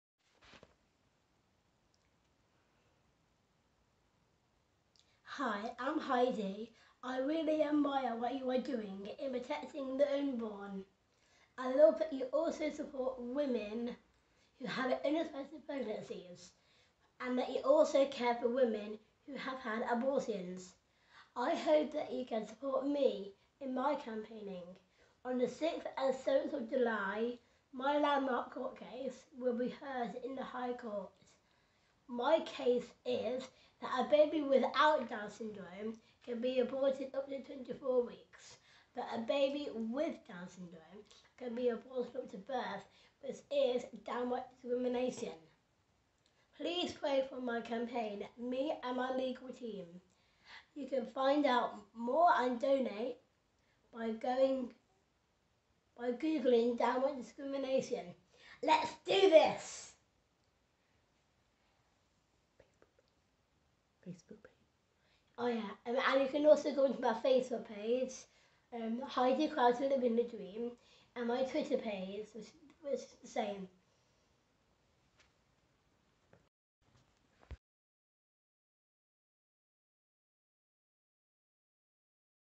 Heidi Crowter - LifeStream 21
It was such a joy to have Heidi join us for LifeStream 21.
Here is her special message to us all!
Go Heidi!!

Heidi Crowter, a 25-year-old woman from Coventry who has Down's syndrome, together with Máire Lea-Wilson from Brentford, West London, whose twenty-three-month-old son Aidan has Down's syndrome, are challenging the UK Government over a disability clause in the current law. A landmark case against the UK Government over the current discriminatory abortion law that allows abortion up to birth for Down's syndrome will be heard at the
High Court on 6th July.